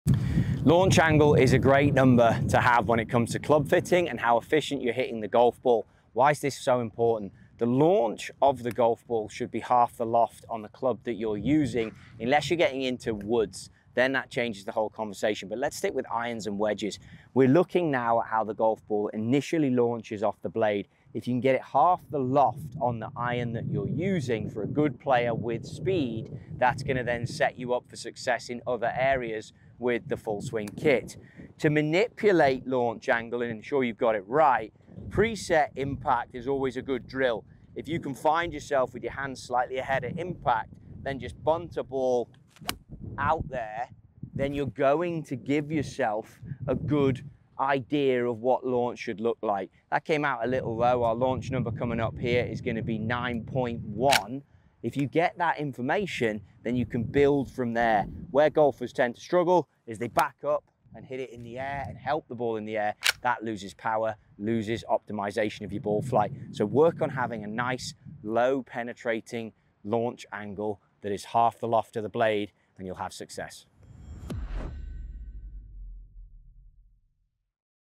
⛳ How Launch Angle Affects Your Shot | Full Swing Data Dive
⛳ Learn how launch angle changes the way your ball flies! This Full Swing Data Dive breaks down what your numbers mean and how to optimize your shots for more distance and accuracy.